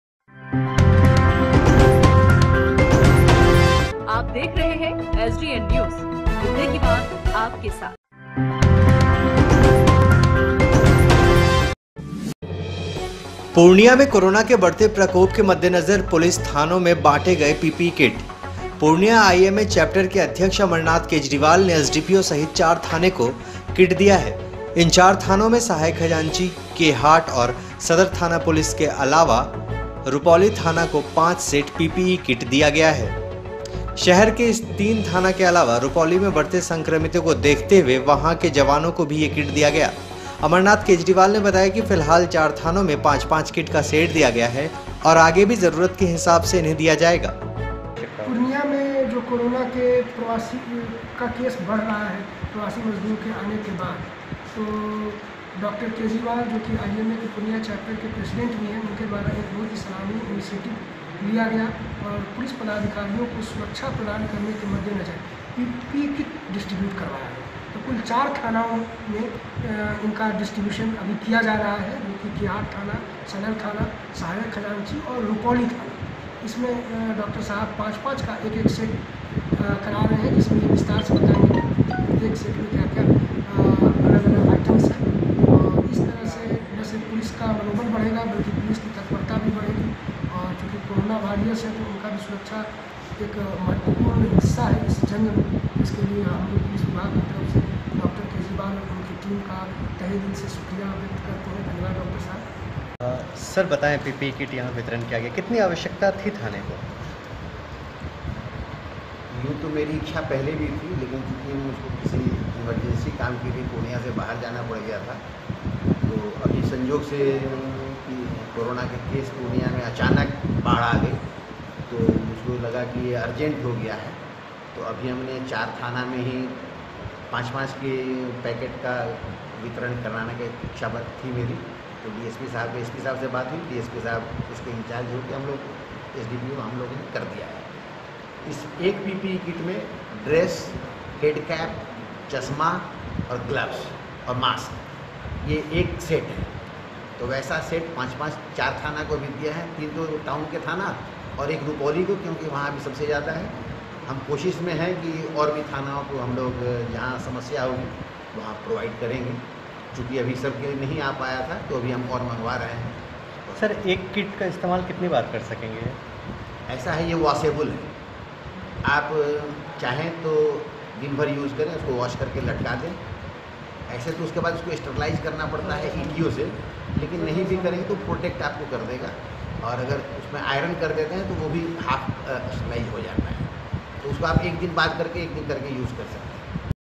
पुर्णिया में कोरोना के बढते प्रकोप के मद्देनज़र आईएमए अध्यक्ष ने पुलिस थानों में बांटे पीपीई किट
पुर्णिया में कोरोना के बढते प्रकोप के मद्देनज़र पुलिस थानों में बांटे गए पीपीई किट । पुर्णिया आईएमए चैप्टर के अध्यक्ष अमरनाथ केजरीवाल ने एसडीपीओ सहित 4 थाने को किट दिया । इन चार थानों में सहायक खजांची, के हाट और सदर थाना पुर्णिया के अलावा रुपौली थाना को 5 सेट पीपीई किया दिया गया । शहर के इस तीन थाना के अलावा रुपौली में बढ़ते संक्रमितों को देखते हुए वहां के जवानों को भी ये किट दिया गया है । अमरनाथ केजरीवाल ने बताया कि फिलहाल 4 थानों में 5-5 किट का सेट दिया गया है, और आगे भी ज़रूरत के हिसाब से दिया जाएगा ।